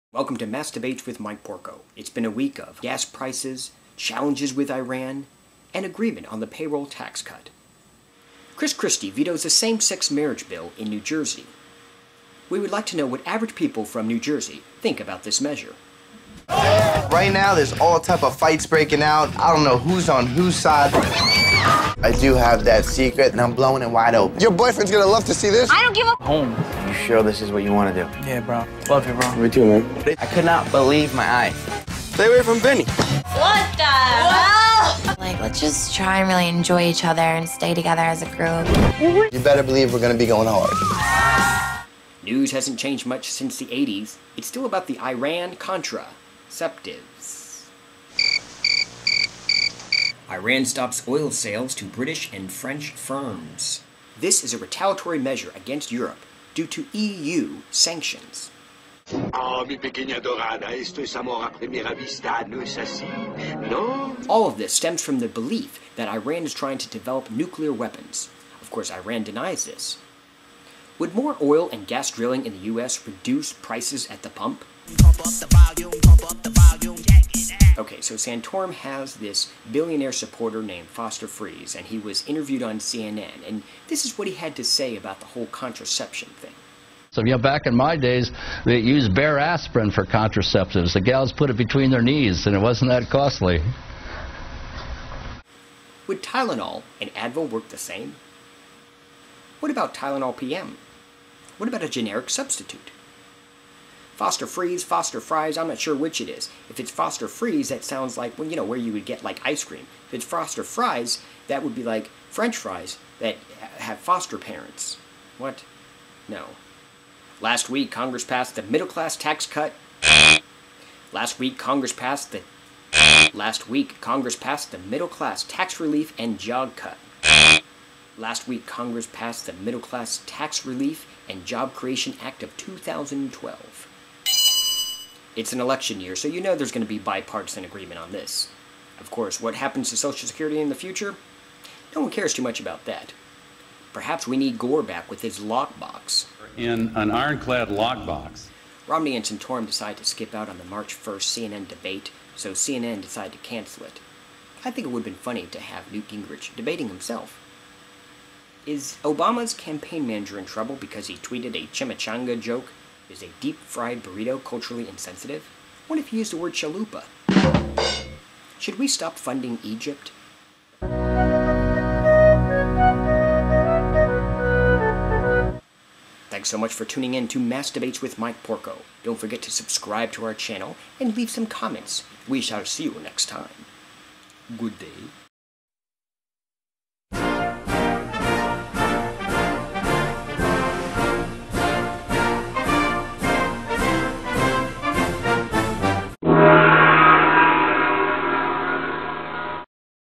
JERSEY ARE YOU SURE? - Mass Debates with Mike Porco
We've got Iran, Payroll tax cut, high gas prices and Christie vetoing same sex marriage in New Jersey. What do average voters in New Jersey think of this?